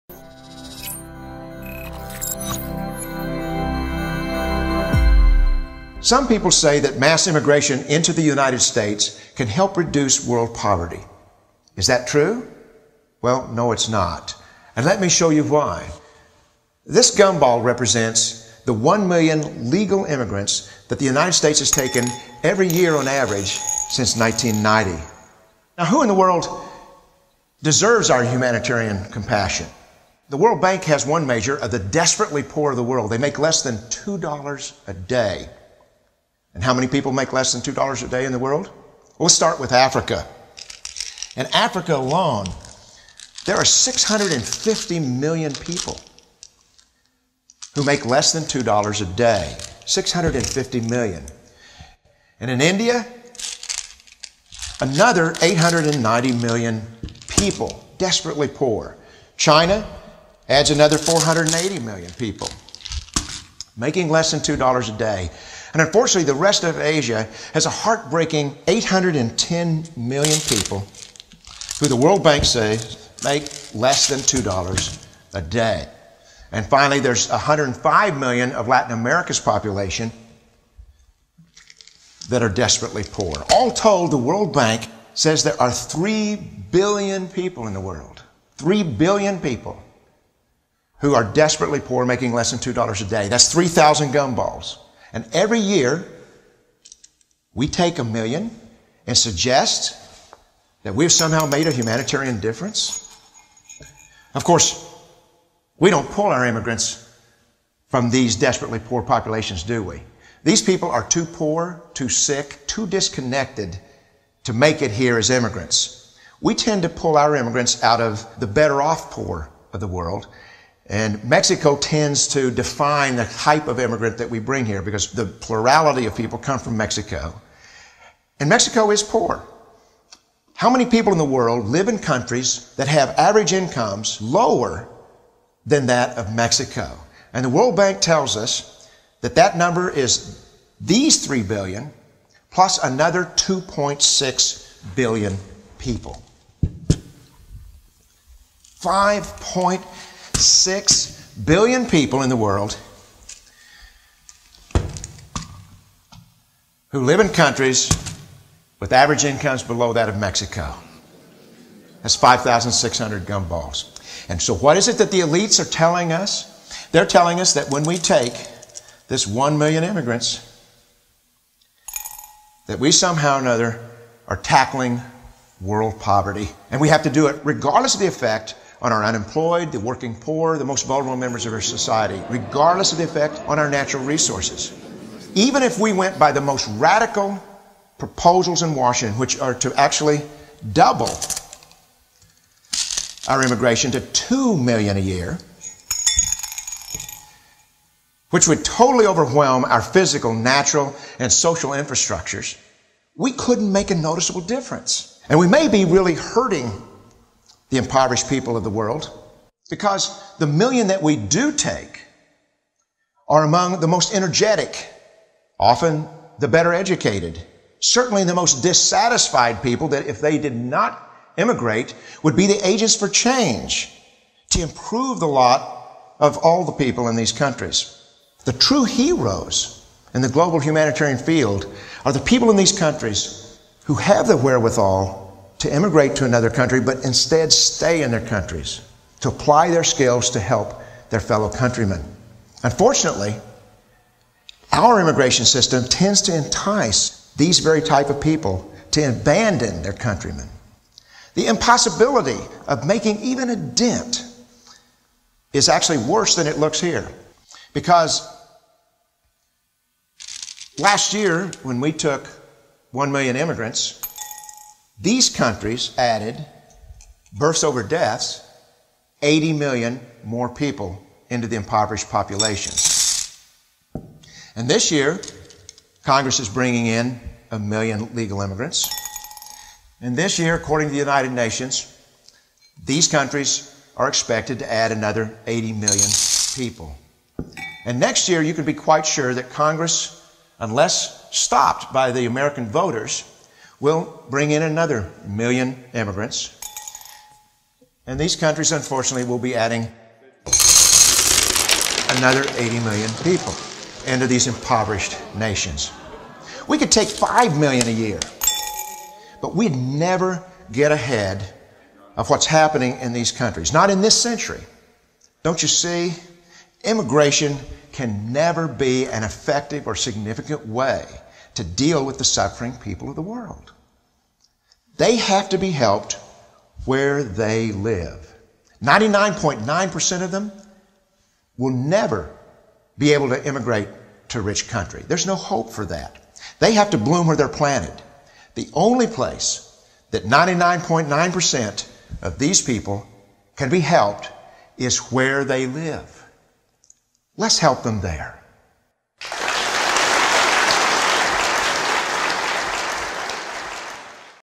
Professor Hans Rosling om Immigrasjon og fattigdom
Immigrasjon er ikke løsningen, forklarer professor Hans Rosling, og viser det ved hjelp av små kuler av tyggegummi.